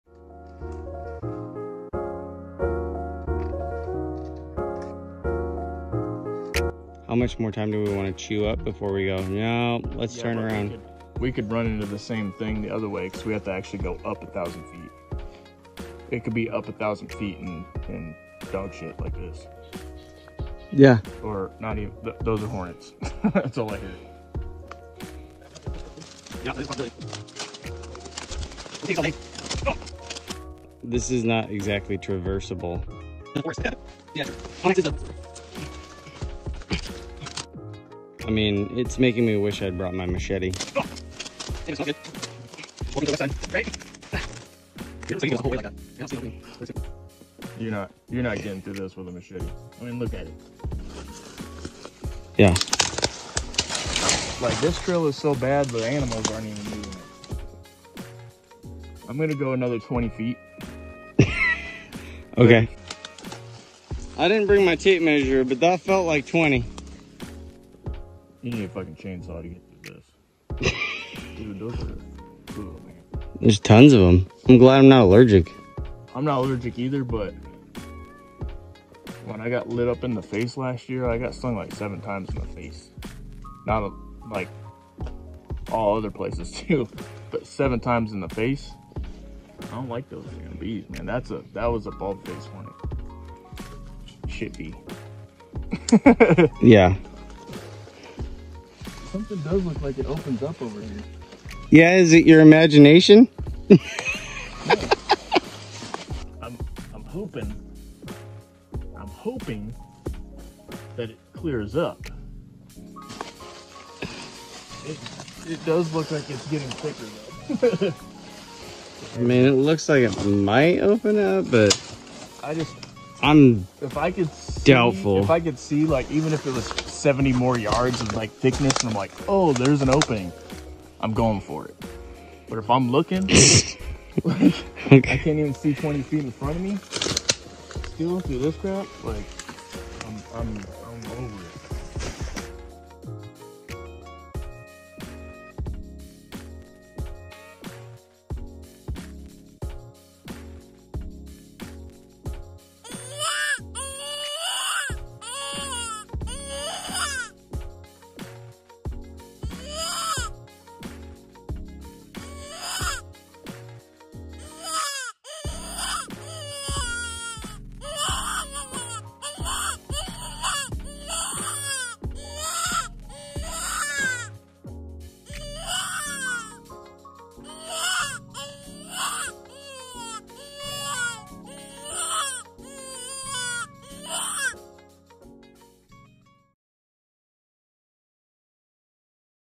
ITs How Far???? Scouting in the jungles of the PNW
In this short video, come along with us, as we discuss the perils of scouting in the jungles of Washington state.